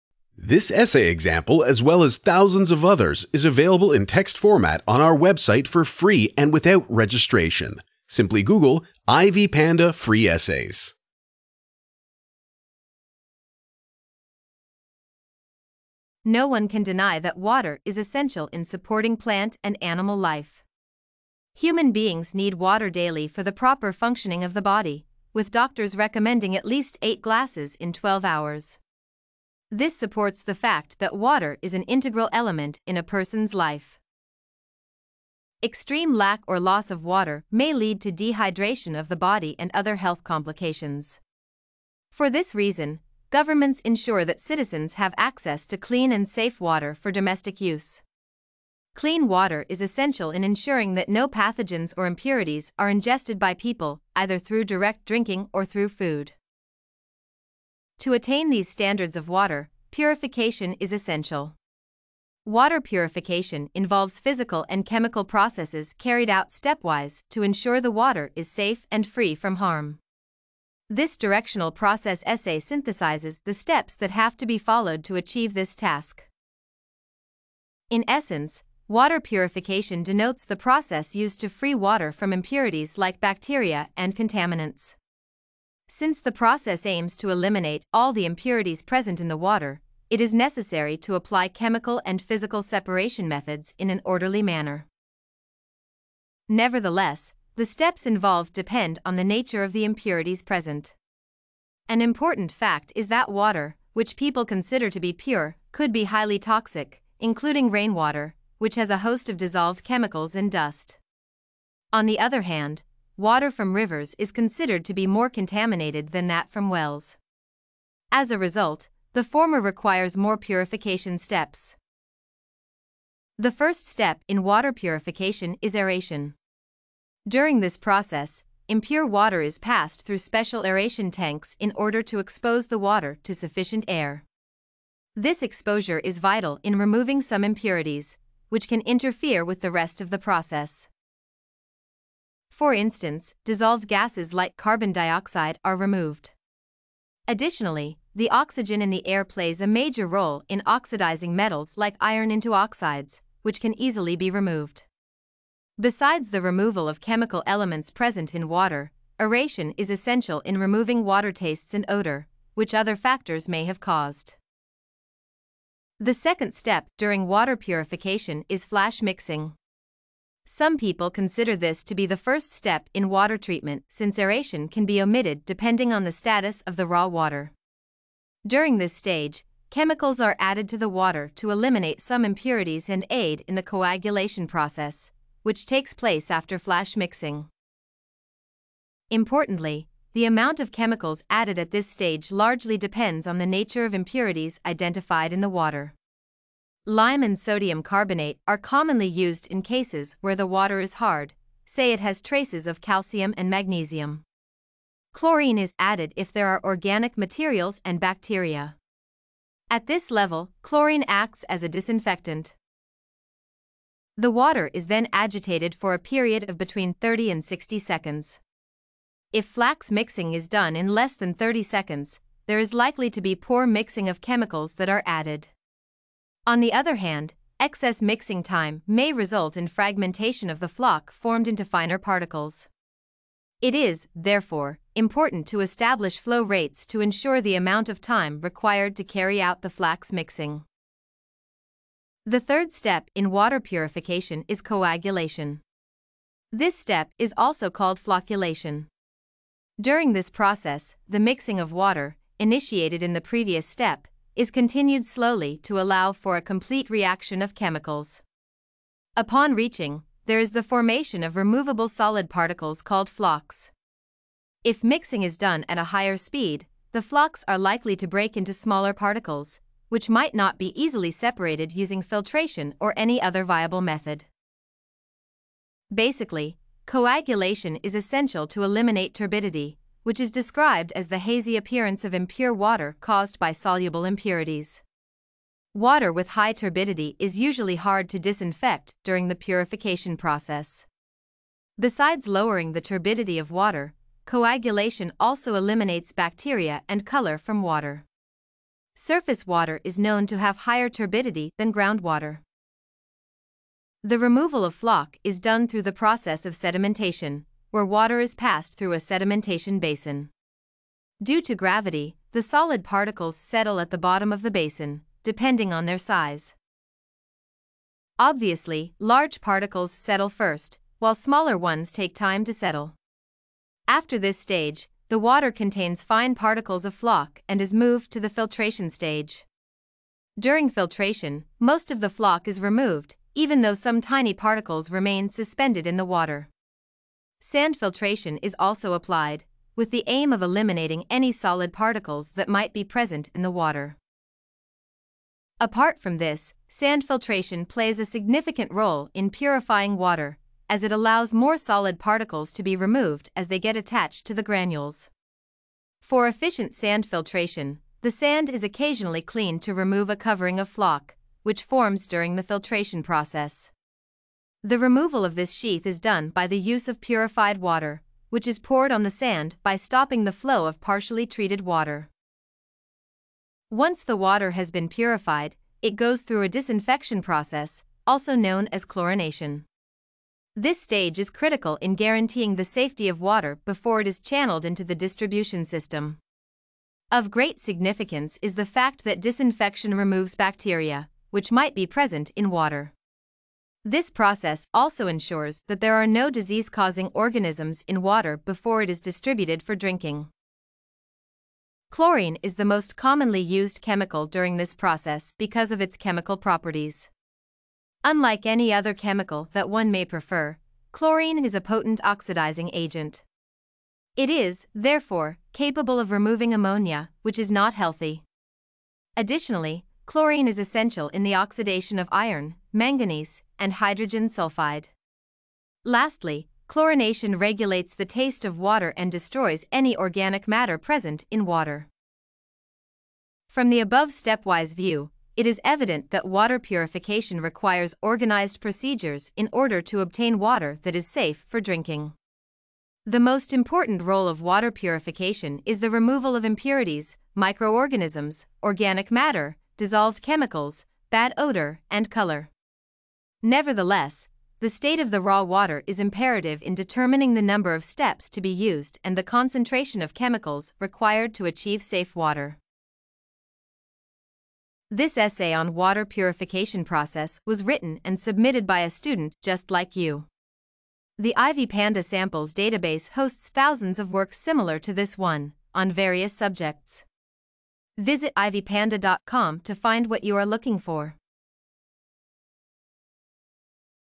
Water Purification Process | Free Essay Example
📝 Water purification involves physical and chemical processes free from any harm. This process essay synthesizes the steps, which have to be followed to achieve this task.

📑 Paper type: Essay
📖 Pages: 4
🔠 Words: 1095
📚 Subjects: Environment, Ecology
⚙️ Language: 🇺🇸 English

🔎 Works Cited:
Wake, Pamela. The Six Steps to Purifying Water. Live Strong, 2010. Web.